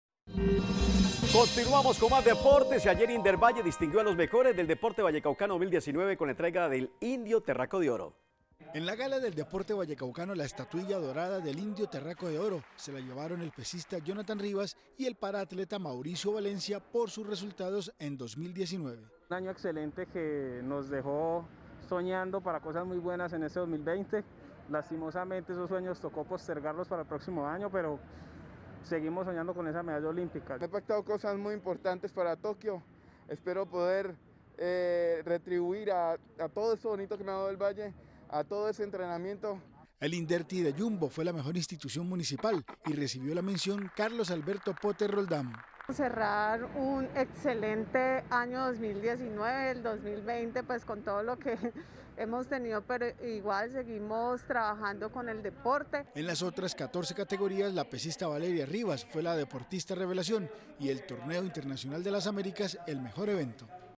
Indervalle distinguió a los mejores del deporte vallecaucano en 2019
Indervalle distinguió a los mejores del deporte vallecaucano en 2019 con la ceremonia de entrega del indio 'Terraco de Oro' en 14 categorías. La ceremonia fue en el auditorio de Telepacífico.